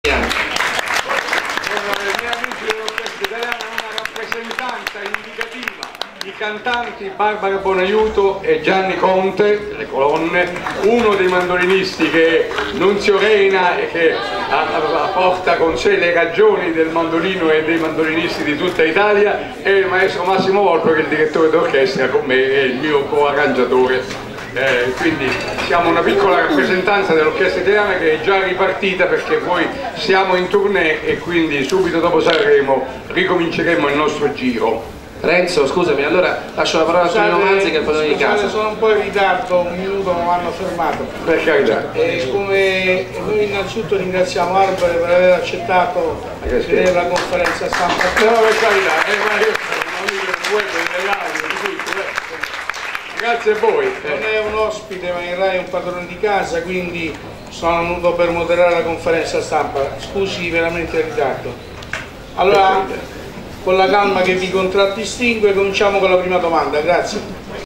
Sanremo Renzo Arbore in Conferenza Stampa
Il video dell'inizio della conferenza stampa di Renzo Arbore al Festival di Sanremo .